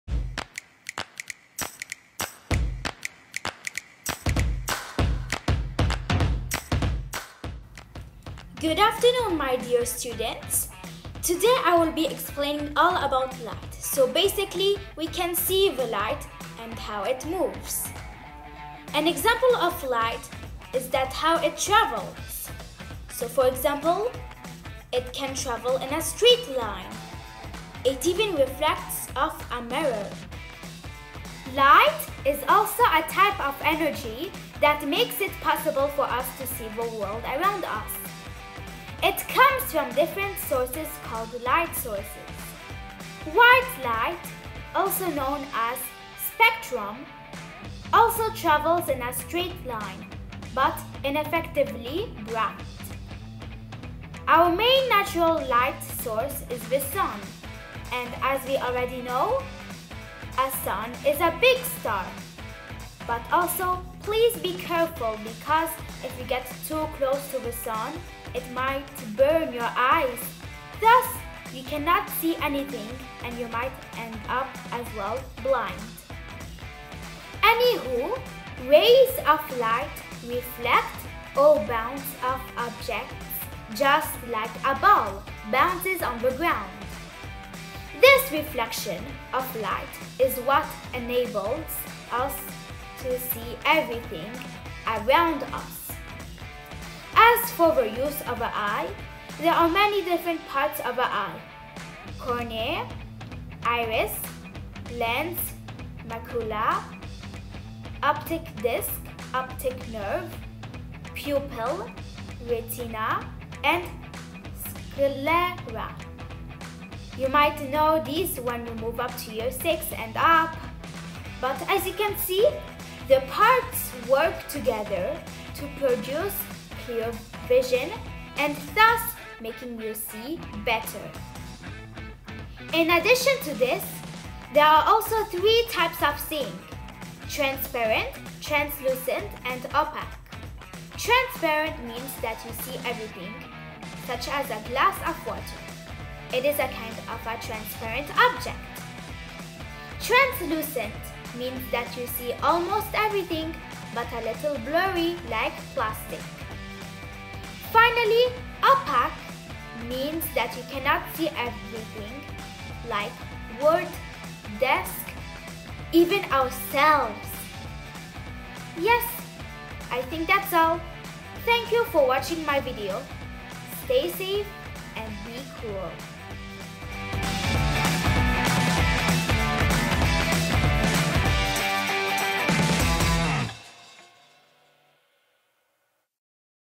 My Science Project About Light ― ScienceWithSalam
This video shows me doing My Science Project About Light. I have made this during school time and at home. Here are the apps I have used:

Clips
iMovie

I have also added some images in order to match some of my information. 😉😉😉

Anyways, I hope you have enjoyed this. 🧪🧪🧬🧬